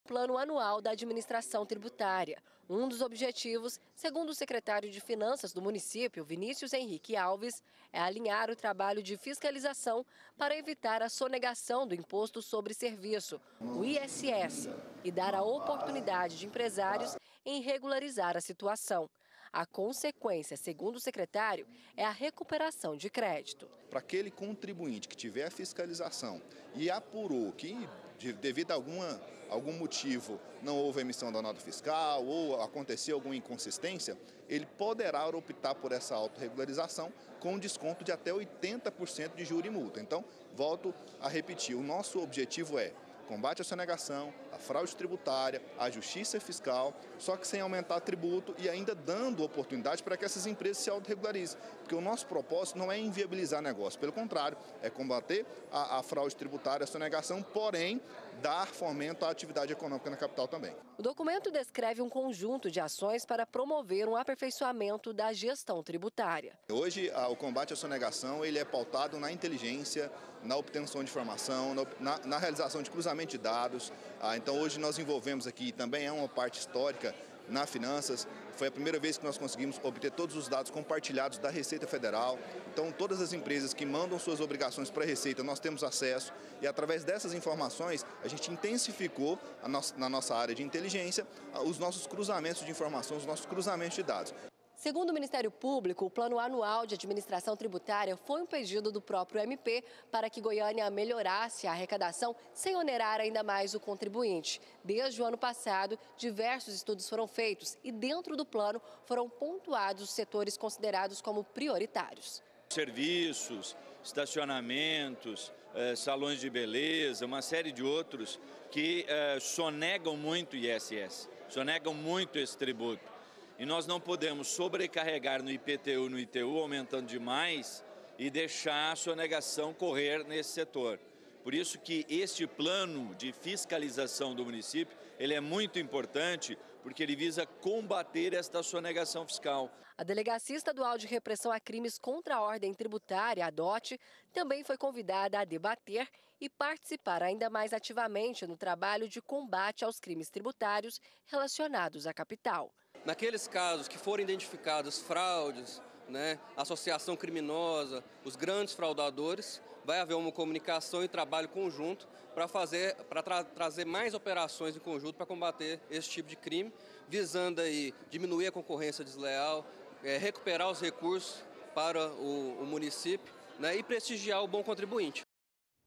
DINHEIRO PÚBLICO: GOIÂNIA FECHA O CERCO CONTRA SONEGAÇÃO DE IMPOSTOS
A PREFEITURA DE GOIÂNIA APRESENTOU HOJE UM PLANO DE ESTRATÉGIA PRA AUMENTAR A ARRECADAÇÃO DO MUNICÍPIO SEM PESAR NO BOLSO DO CONTRIBUINTE. UMA DAS AÇÕES É APERTAR O CERCO CONTRA EMPRESAS QUE SONEGAM IMPOSTOS.